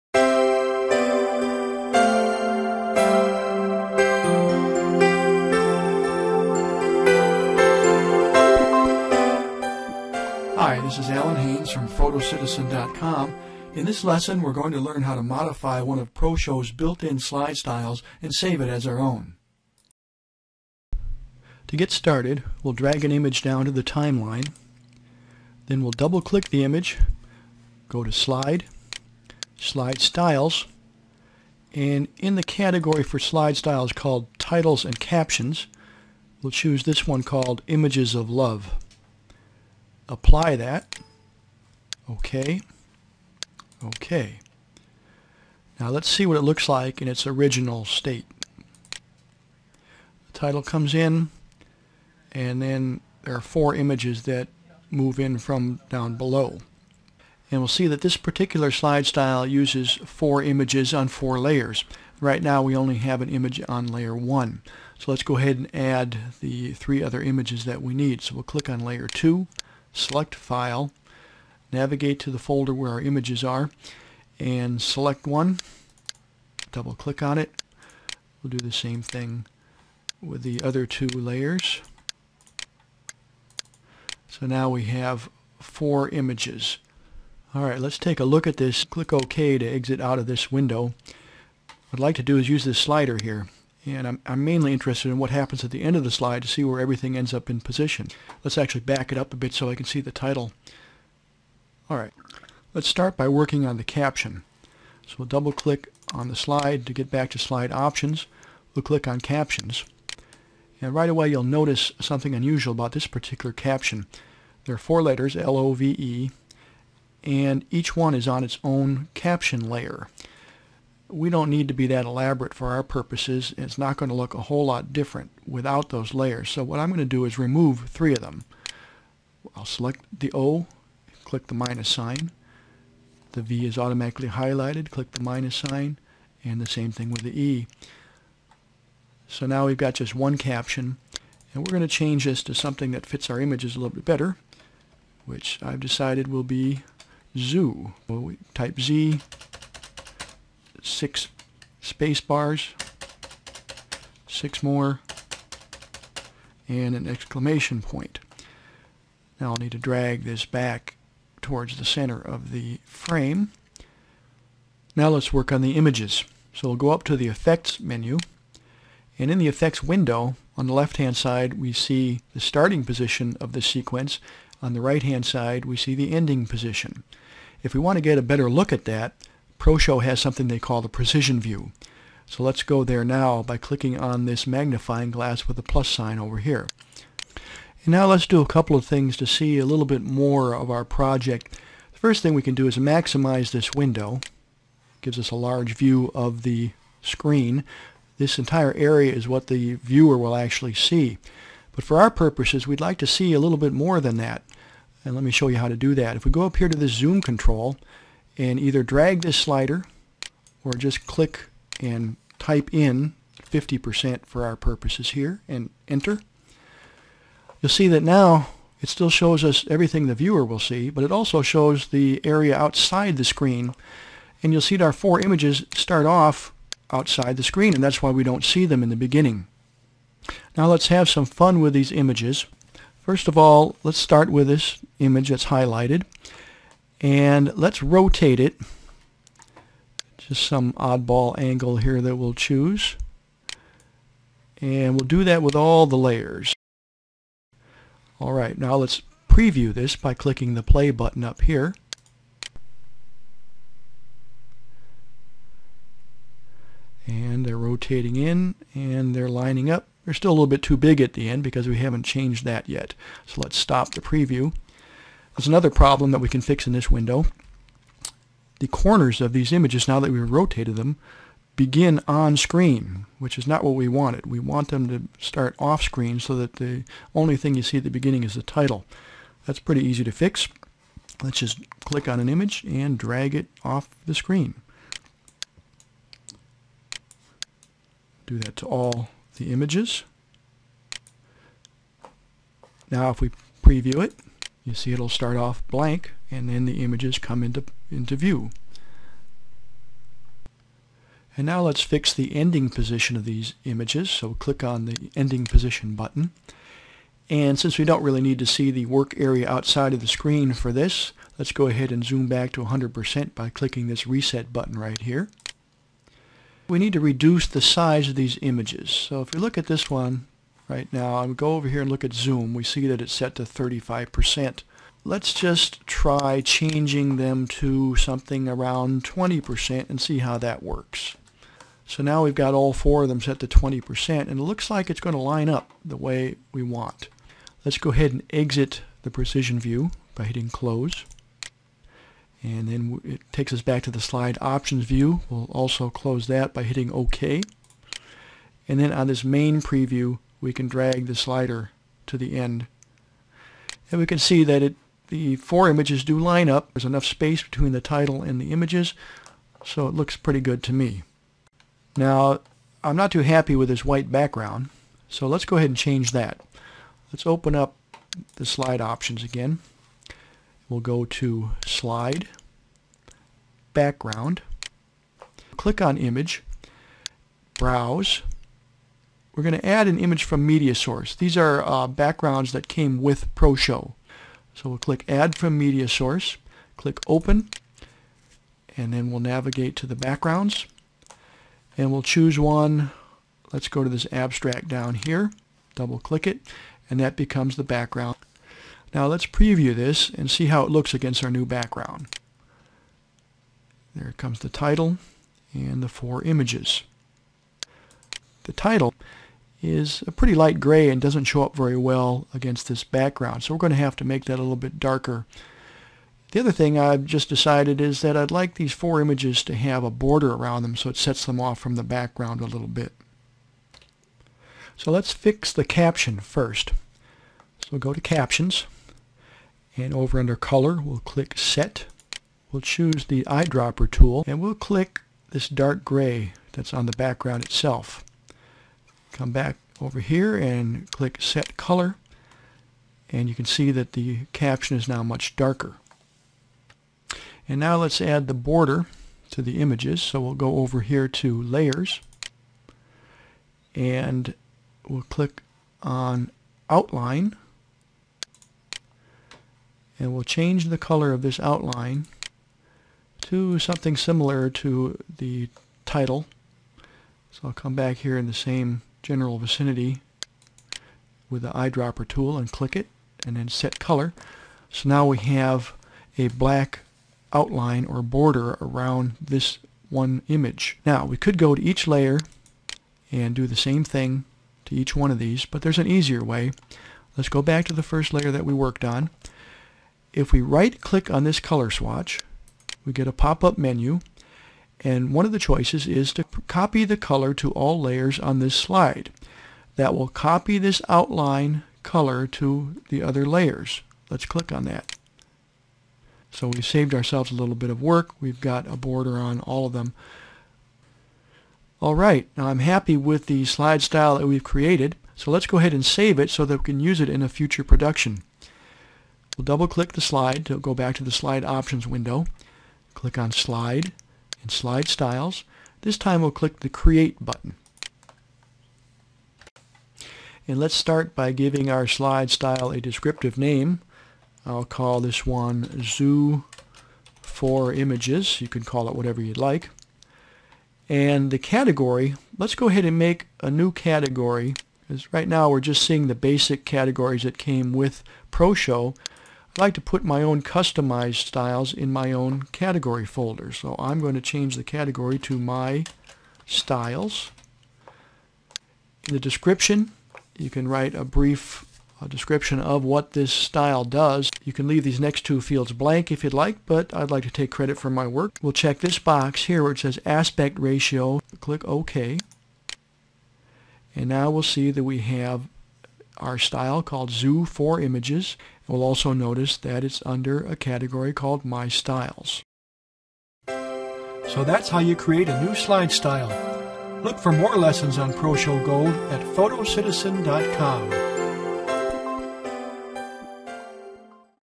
ProShow Gold Advanced Slide Styles Part 3 HD
Learn how to modify a built-in slide style in ProShow Gold 4 and save it as a new slide style.